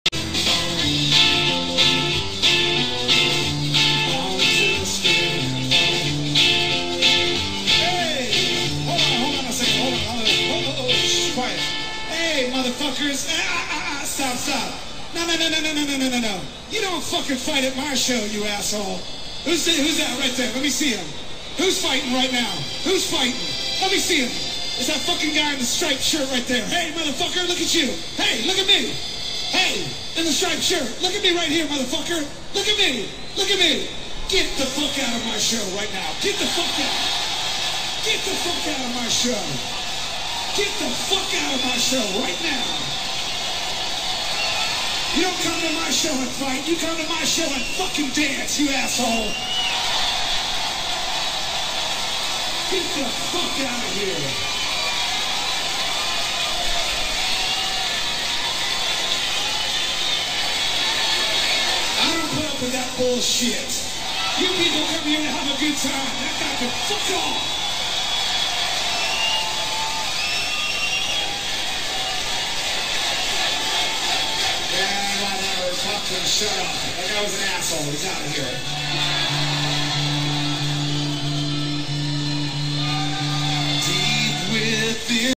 Dave Grohl -PISSED at fan fight 7/12/11 Foo Fighters
At a recent gig in London, Foo Fighters frontman and all round good bloke, Dave Grohl stopped the set to kick out a moron in the crowd that was trying to fight other audience members. Grohl unleashed a highly amusing rant that culminated in him declaring "You don't come to my show and fight, you come to my show and dance, you asshole. Get the fuck outta here!"   Don't be a fighting kind of jerk at a Foo Fighters concert. He wasn't being nasty, he let an asshole know that asshole behavior isn't welcome. Dave Grohl exemplifies what being a star is all about...good music, good guy and good concerts.

Dating right back to his days with Nirvana, Dave Grohl has carved out a reputation as being one of the most geniunely decent people in the filled with subhumans, toads, and trolls cesspool that is the music business. How nice? Well consider a little found poem that ran in the Guardian? a couple of years back under the headline of "Is Dave Grohl the nicest guy in rock?"

In the piece, we get the following anecdote: "Then you have artists like Dave Grohl. My friend Giles has a sister with severe Cerebral Palsy who is a massive Foo Fighters fan. After watching them at Reading one summer, Giles and his sister waited outside the artist area hoping to get a glimpse of Grohl. Sure enough, eventually he appeared, strolling on his own across the field. As he saw Giles's sister, he walked up to her and asked: 'Aren't you the girl who has sent us pictures of you dancing to our music?'

"As she nodded yes, he gave her a big hug with a beaming smile and spent 20 minutes chatting to her before eventually wandering off to the canteen.

"A few weeks later, Giles and his sister went to see Queens of the Stone Age (with Grohl sitting in on drums) at the Astoria. After getting a pass to watch the gig from the side of the stage by a merchandise seller (could he possibly be the nicest merch guy in rock?), they again saw Grohl beaming as he saw her standing there during the gig. After the set, he had another chat with her and invited her to the following day's video shoot, where he spent time with her between takes.

"All this happened without a camera—apart from Giles's—or publicity person in sight. Maybe he just realised what a huge impact it would have on her life and self esteem."

Evidently though, even nice guys have their breaking point. Check out the following clip, shot in London on July 11 at the iTunes festival, in which Grohl makes the Flaming Lips' Wayne Coyne look like Lou Reed on the rag. And, might we say, with very good reason.

Ask yourself this: what kind of flaming dink do you have to be for Dave Grohl to call you an asshole? And if you're answer is "think a Frankenstein monster made up of the worst parts of Van Morrison, Axl Rose, and Fred Durst" you're on the right track.